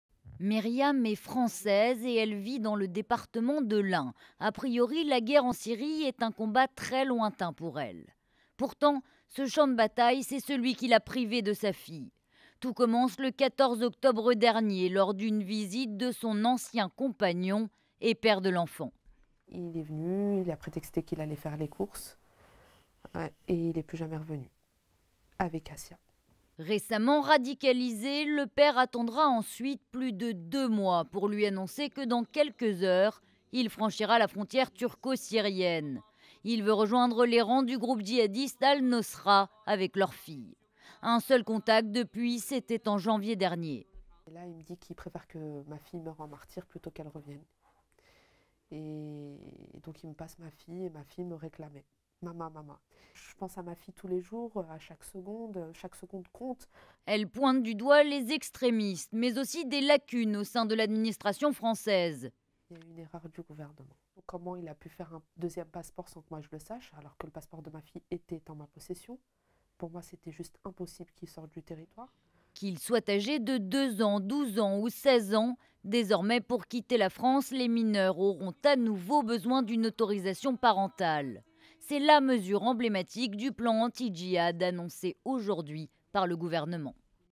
Djihad en Syrie : Meriam s'inquiète pour sa fille
Le ministre de l'Intérieur, Bernard Cazeneuve, va présenter un plan pour dissuader les jeunes de partir faire le djihad. Ce mot, Meriam le connaît que trop bien.
Son ancien compagnon a rejoint les rangs des combattants en Syrie. Problème : il y a emmené sa petite fille âgée de seulement un an et demi.